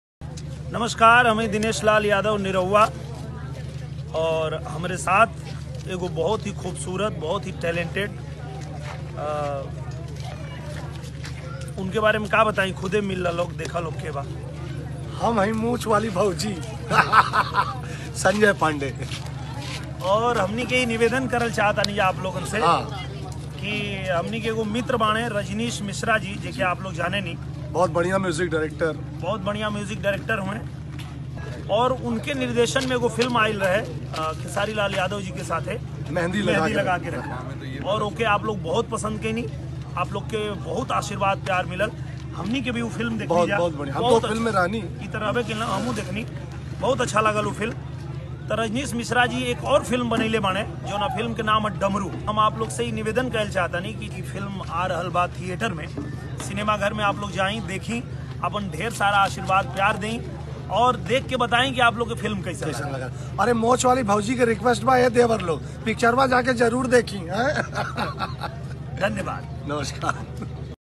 खेसारी लाल की इस फ़िल्म को देखने की अपील कर रहे है निरहुआ
Jubliee star Dinesh Lal Yadav nirahua requesting to his fans to watch Khesari Lal' Damru.